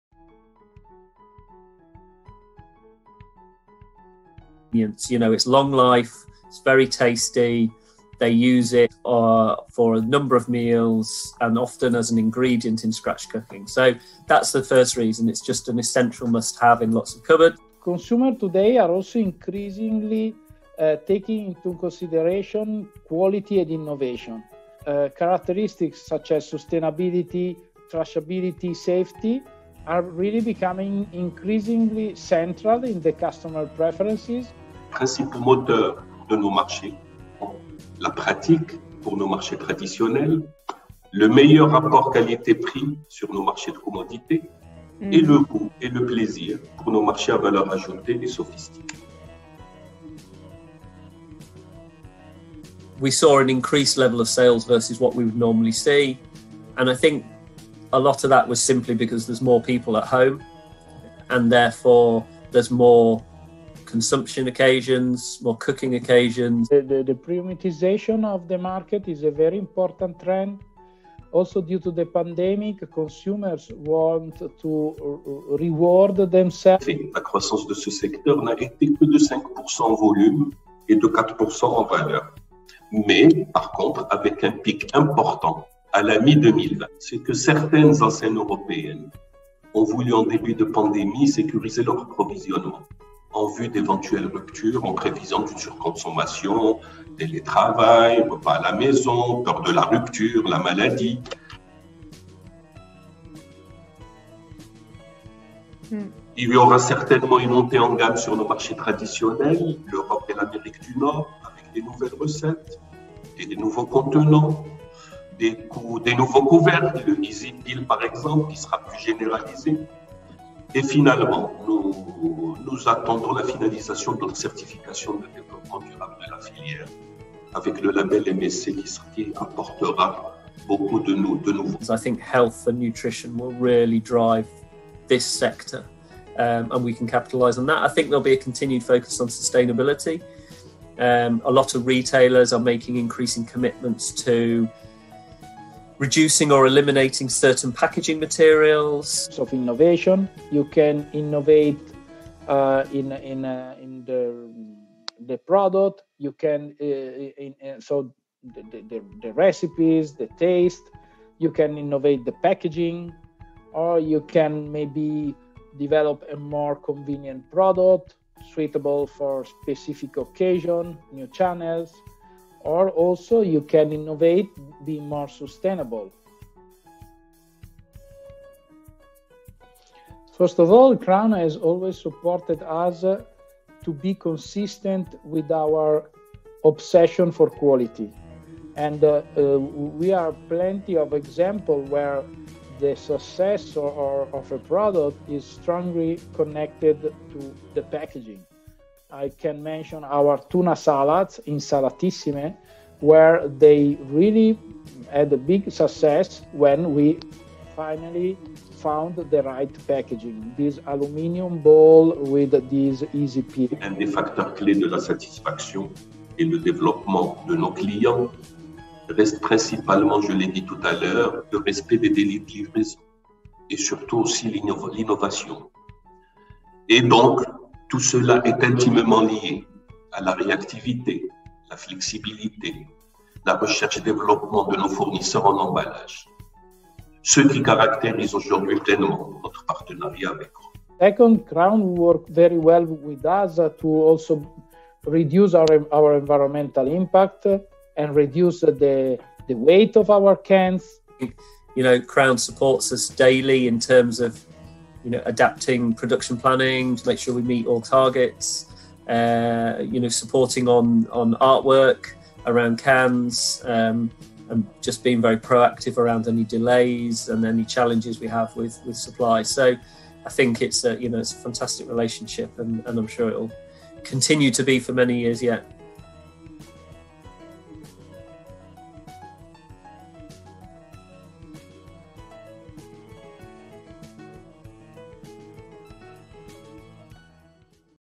Canned Fish: the convenient and nutritious alternative
Hear what the experts of the canned fish industry have to say when it comes to benefits, trends, COVID impact and benefits of working with Crown.